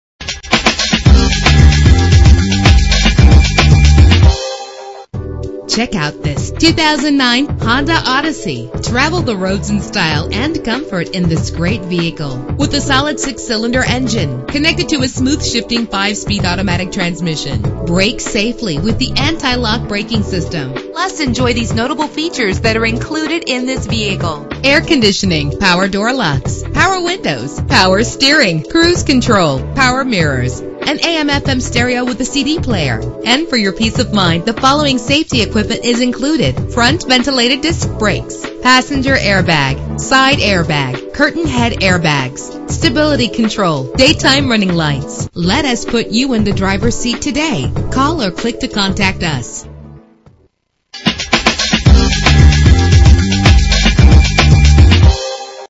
2009 Honda Odyssey - Manassas VA
Year: 2009
Make: Honda
Model: Odyssey
Trim: LX
Engine: 3.5 liter 6 cylinder 24 valve
Transmission: 5-Speed Automatic
Color: Dark Cherry Pearl
Mileage: 33609
Address:
8710 Kinchloe Dr.